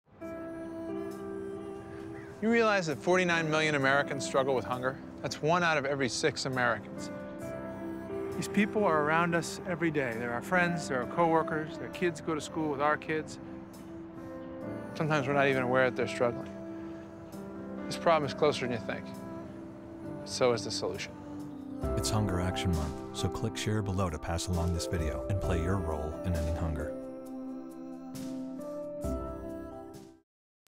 Feeding America Hunger Action Month PSA (HD)
A Feeding America Public Service Announcement with Ben Affleck, as a part of Hunger Action Month.